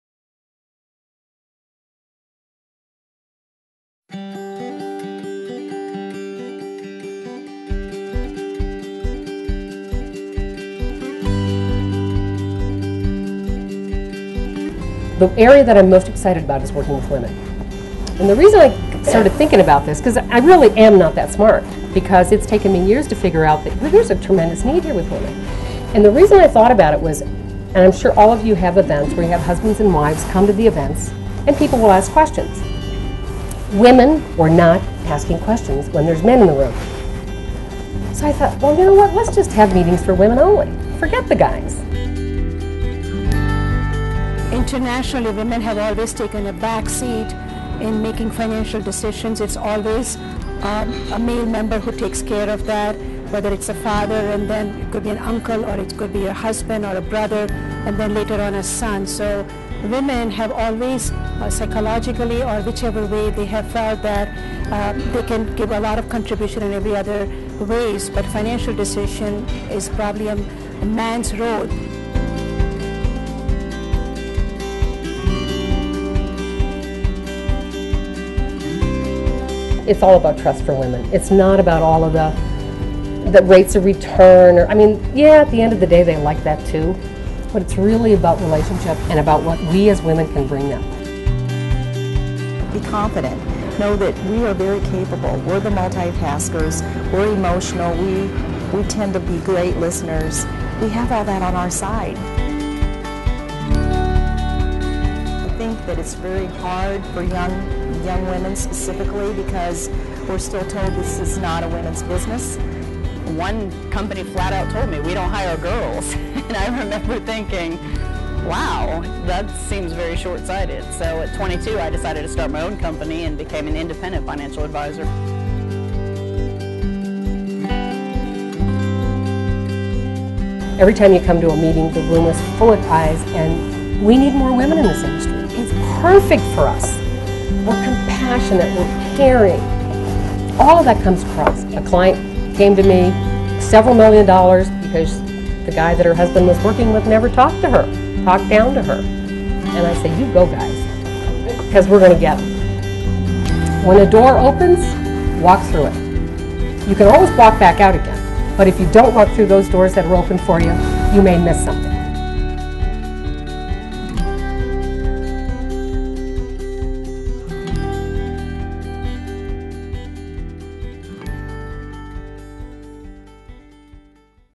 Download Securities America - Super Women Summit
Footage and commentary from the Securities America Super Women Summit.